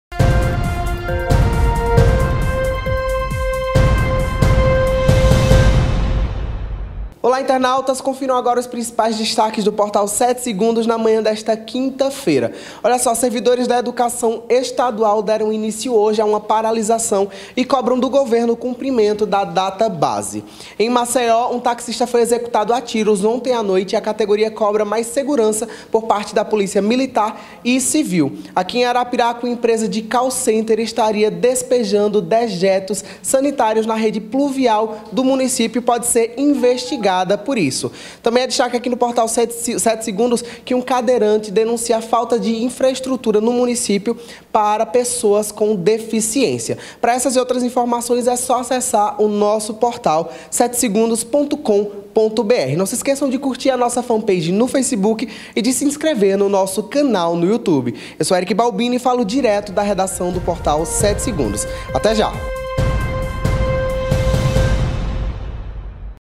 7 em Destaque
Confiram os destaque do Portal 7 Segundos desta manhã: servidores da educação paralisam; taxista é assassinado; falta de acessibilidade; empresa lança dejetos.